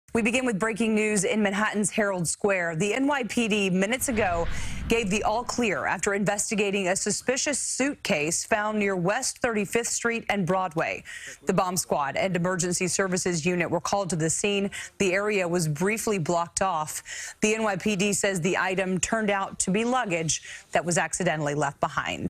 Luggage Prompts Suspicious Package Scare In Herald Square
Traffic in one of Manhattan's busiest neighborhoods was brought to a standstill for over an hour as police responded to reports of a suspicious package Saturday evening.